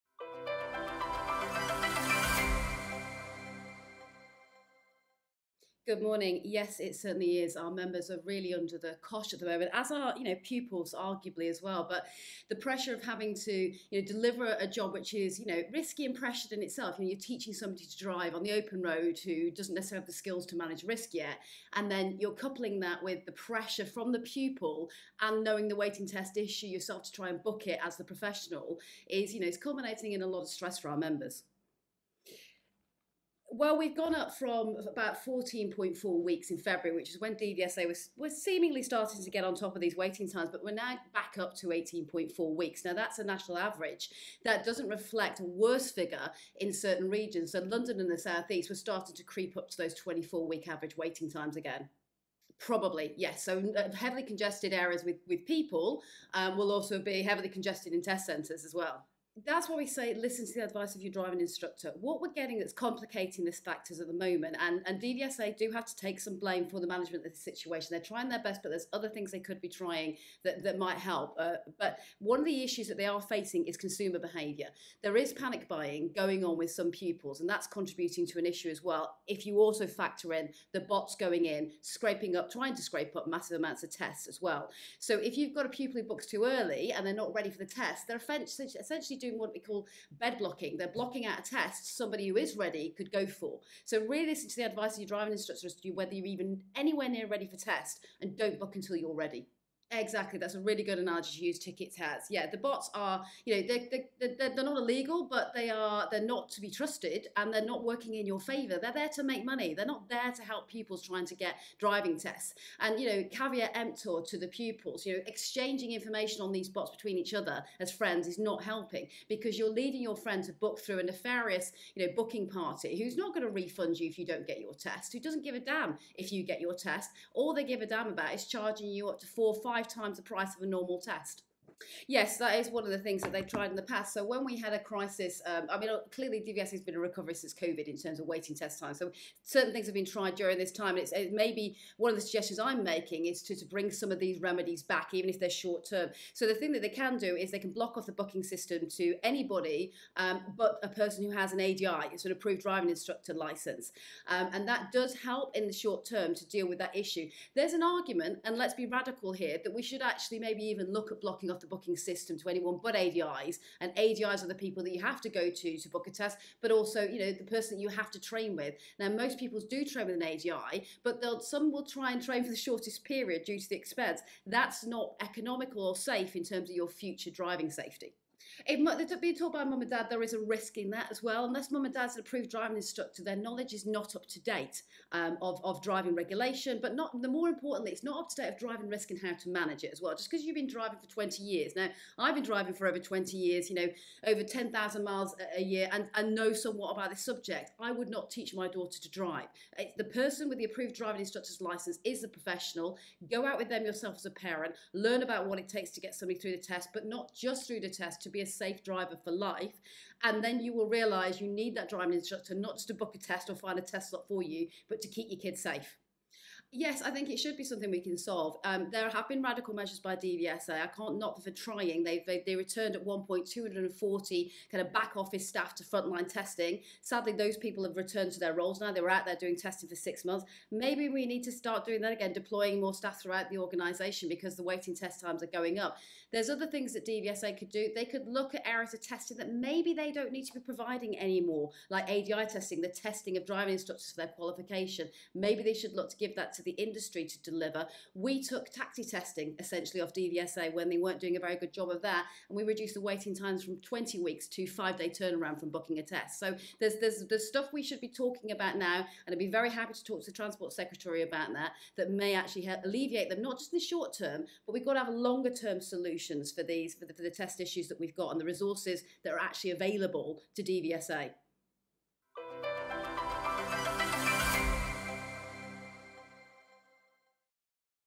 DIA CEO Carly Talks Test Waiting Times on BBC Radio Kent
Here's our CEO Carly on BBC Radio Kent yesterday talking about test waiting times. Apologies, we only have one side of the interview here; we're trying to get the whole audio of the piece from BBC. And apologies from Carly as well for the 'pulled through a hedge look' she's sporting, she'd just run in from a run to do this breakfast time slot.